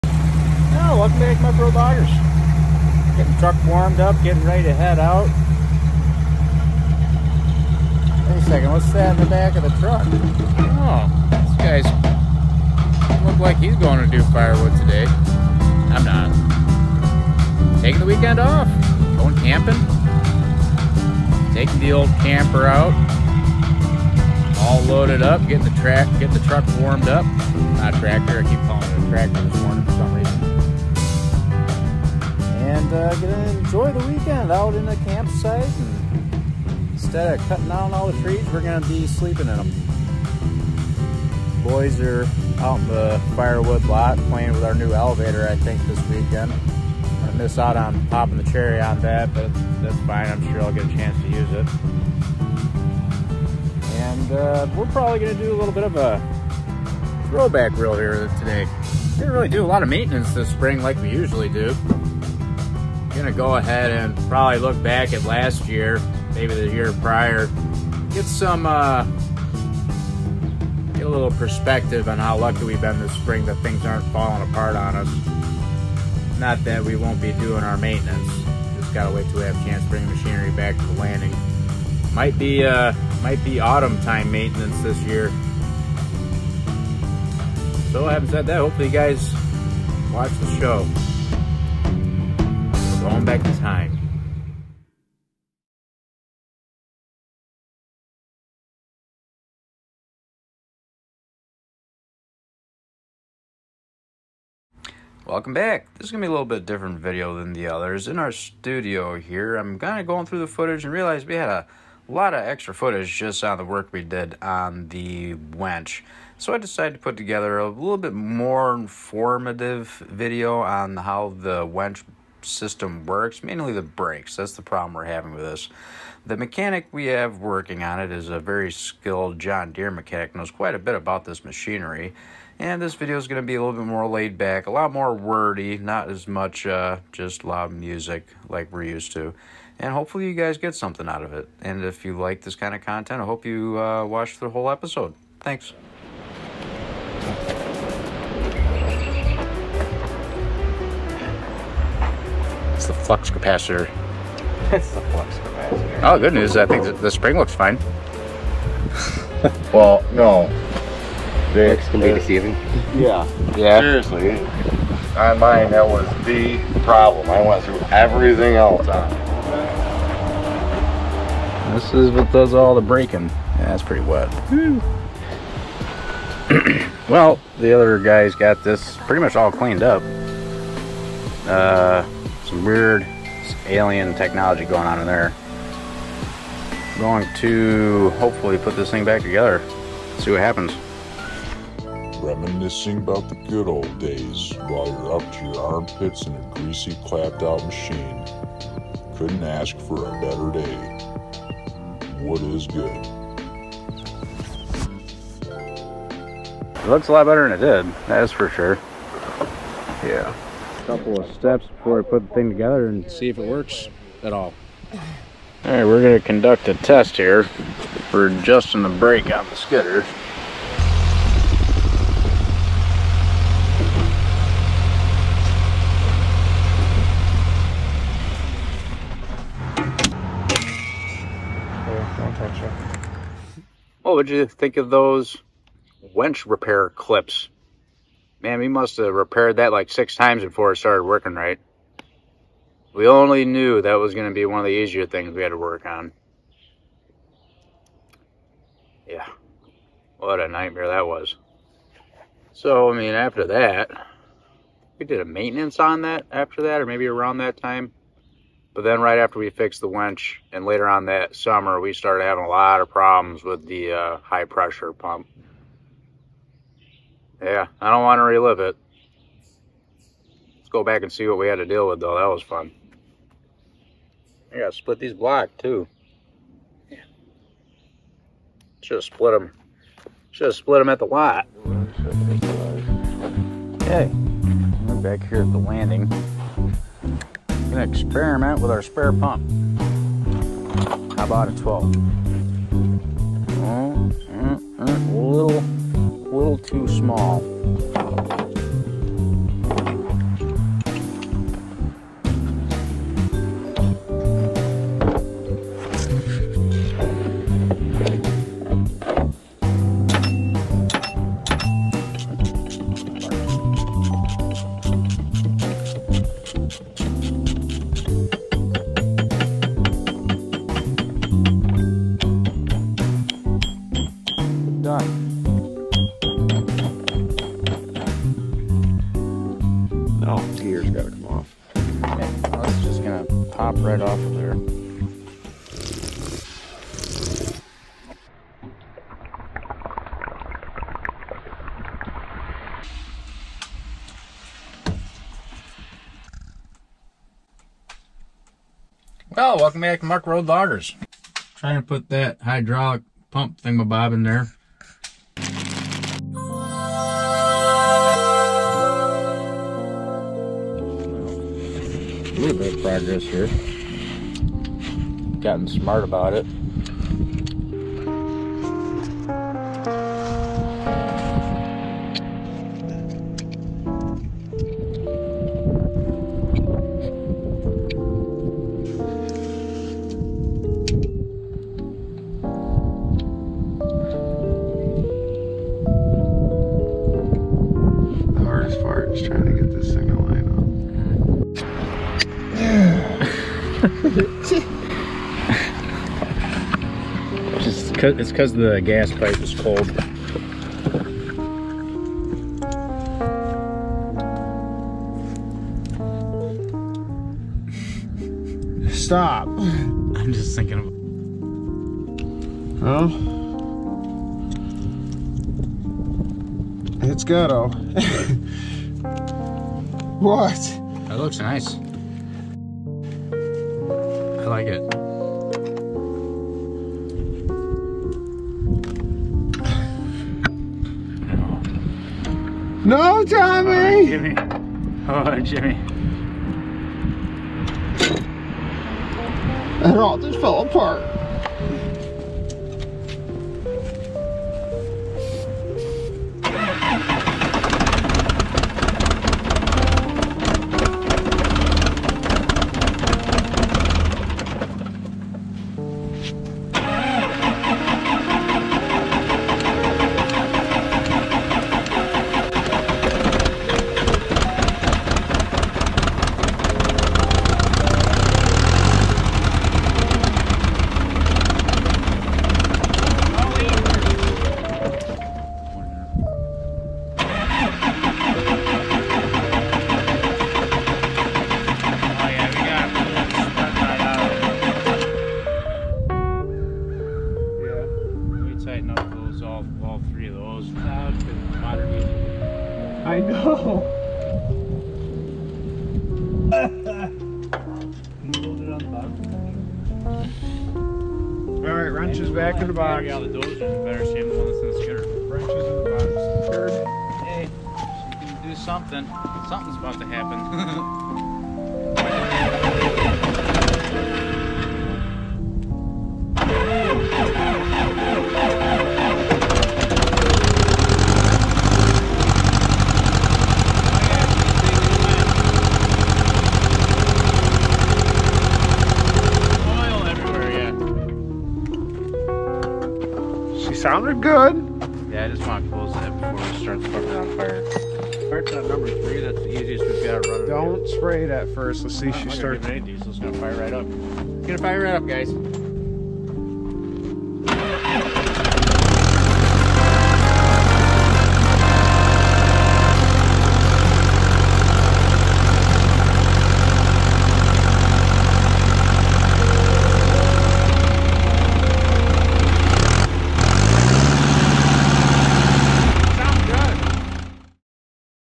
Summer Vacation Through back.
In Summer Vacation Through back.we are taking a look back at some past repairs to the skidded.